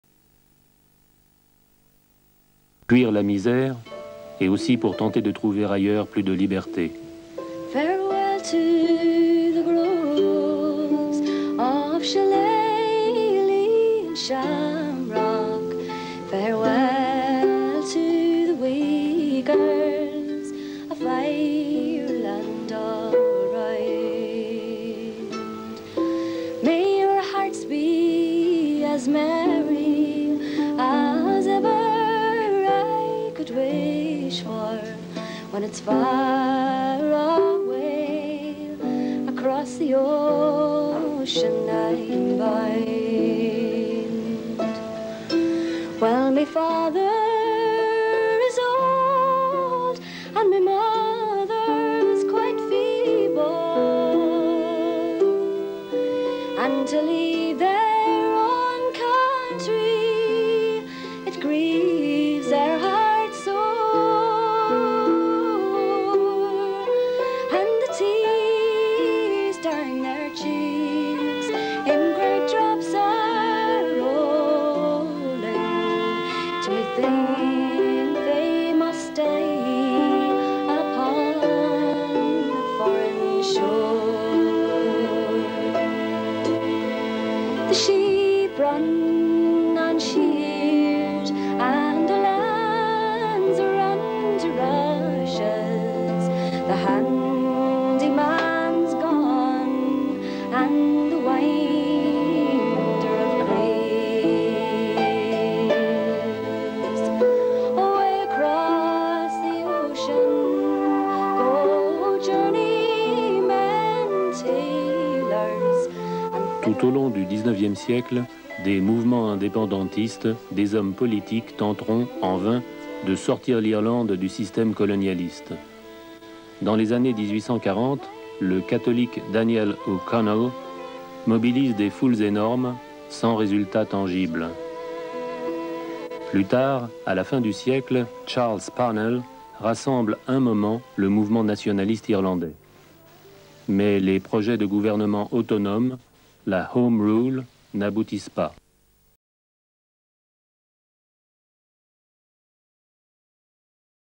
Deanta - French TV 1994
County Derry Irish trad sensations Deanta, featuring vocalist Mary Dillon, doing their thing on French TV.